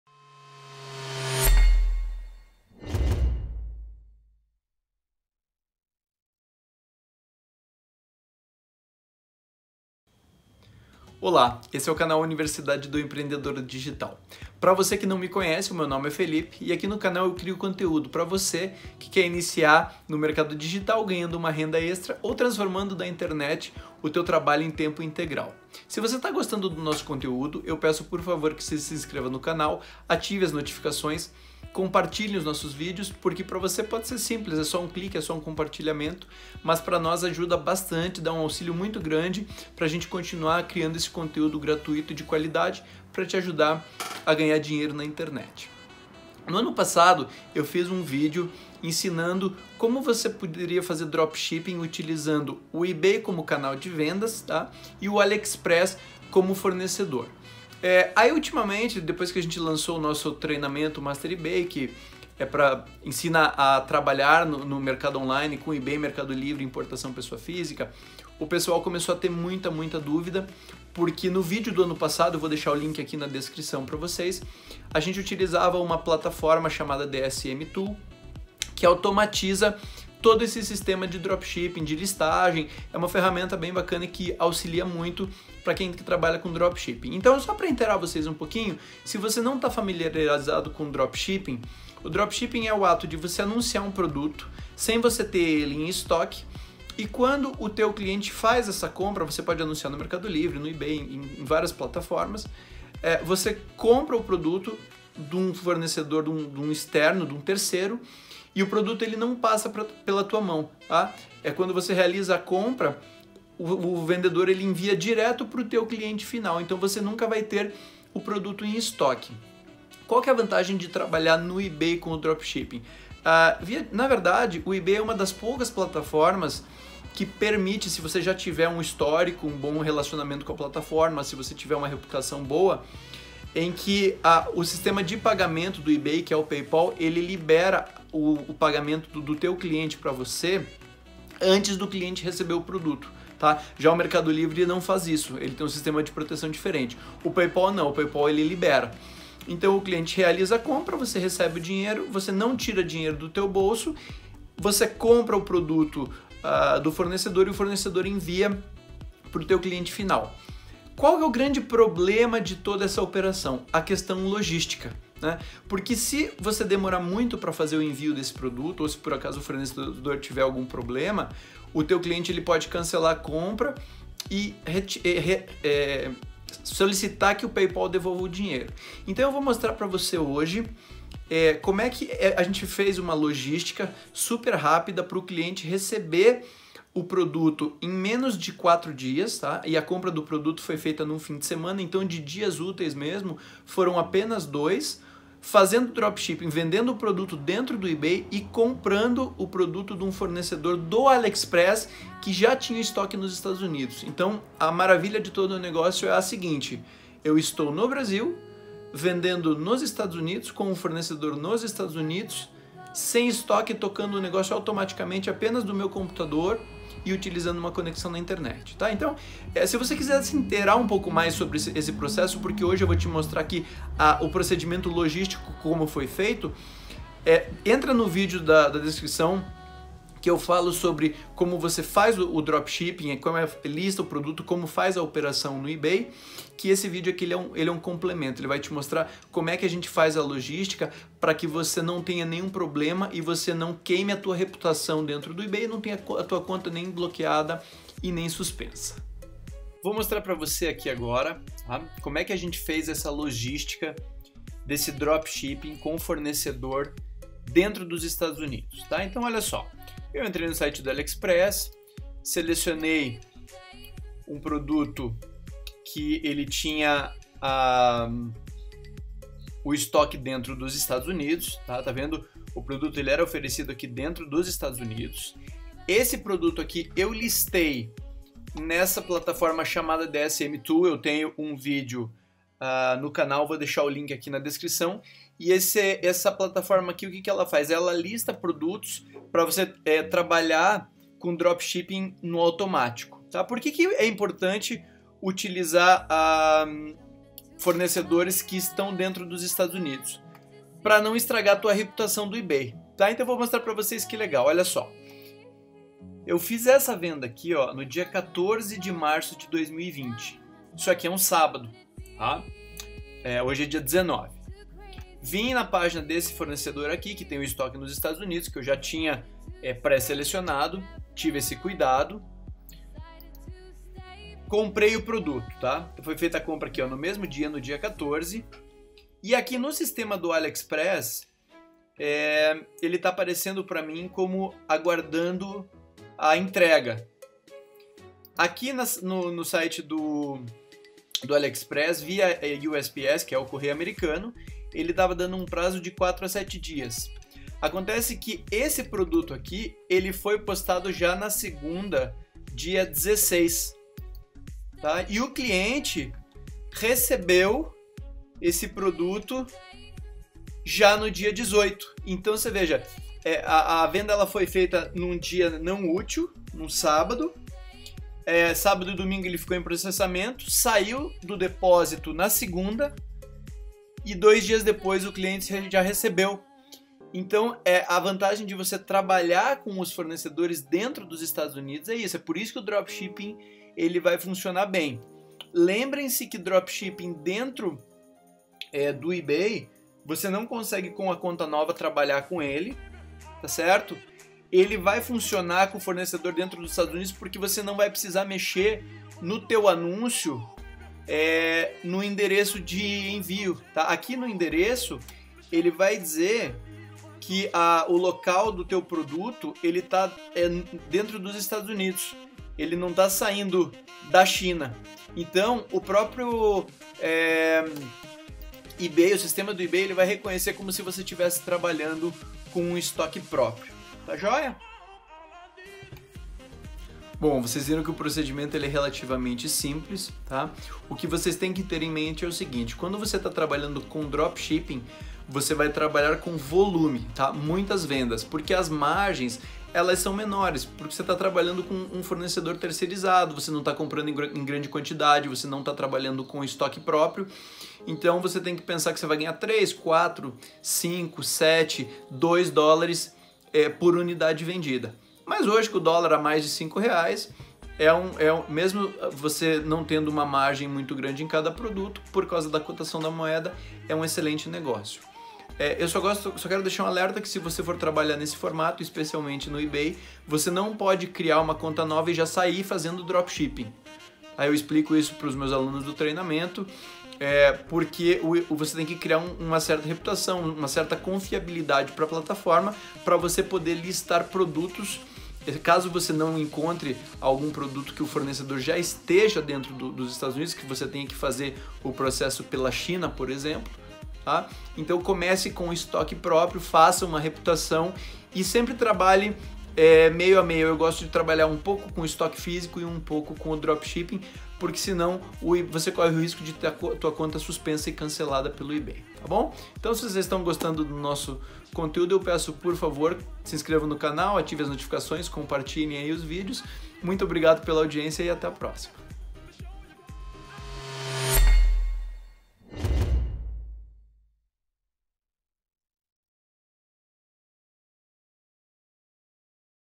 Dropshipping Aliexpress Ebay- Cliente recebeu o produto em 4 dias- Ganhar dinheiro na internet
👉 Treinamento Master E-Bay + Importação Pessoa Física, comece a ganhar em dólar hoje

Neste vídeo eu vou te mostrar como você pode vender no E-bay utilizando o Aliexpress como fornecedor com estoque dentro dos Estados Unidos. Vou te mostrar como foi feita a logística para que você entenda que é perfeitamente possível trabalhar com dropshipping no E-bay desde que respeite os prazos. Desta forma fica muito fácil ganhar dinheiro online com pouco capital. Comece agora. Ganhe dinheiro online a partir de hoje.